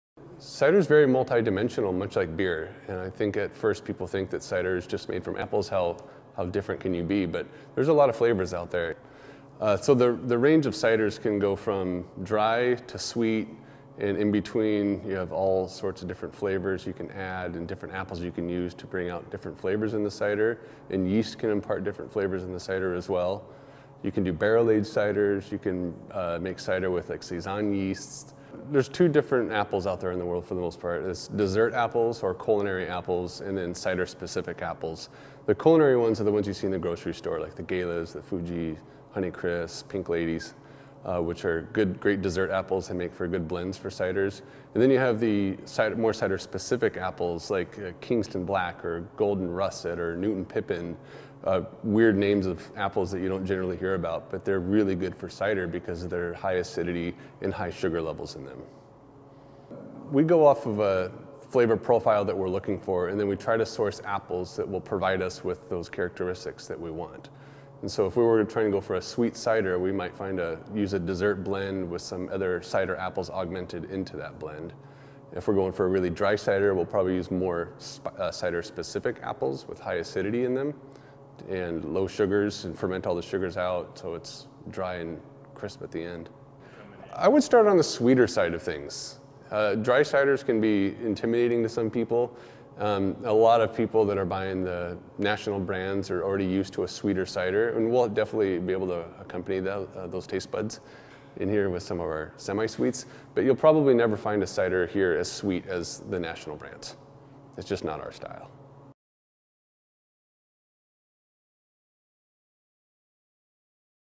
Co-owner of new Bellingham cider company explains the basics of this popular adult beverage
Chris Noskoff, co-owner of Lost Giants Cider Company, talks about cider basics on Thursday, June 14, at Lost Giants Cider Company in Bellingham.